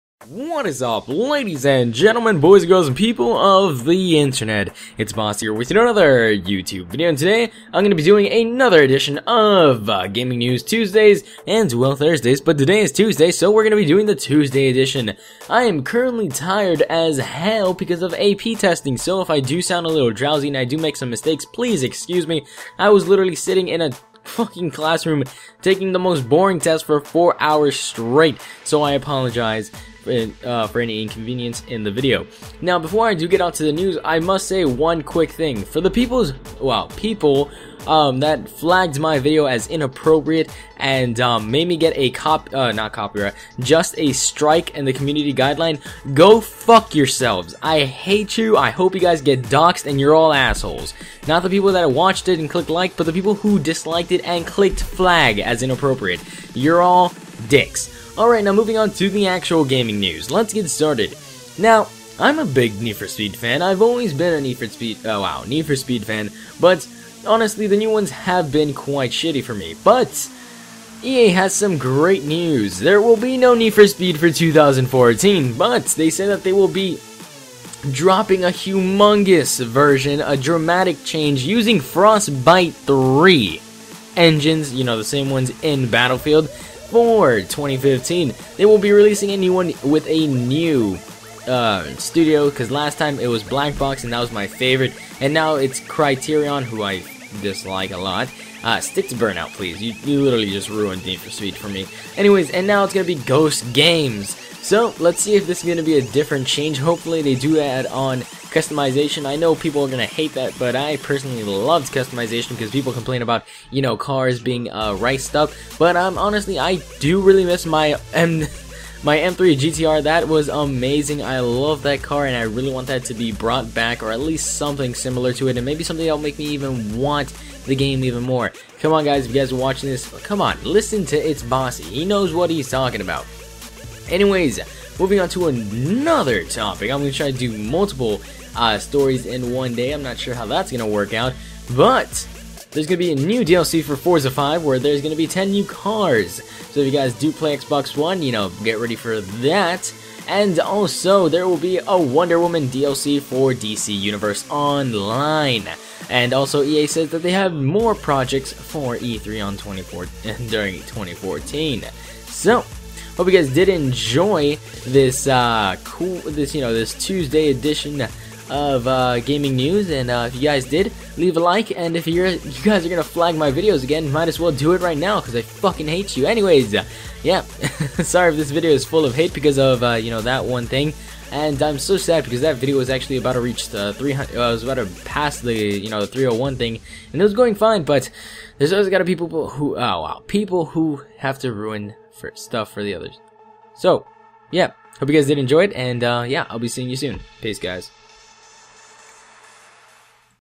ItsBassi Gaming News: No NFS in 2014, Wonder Woman DLC, & New Forza DLC
Song(s):

♆ Wanna get partnered? Come join me in NecessaryGaming.
Partner with NG today!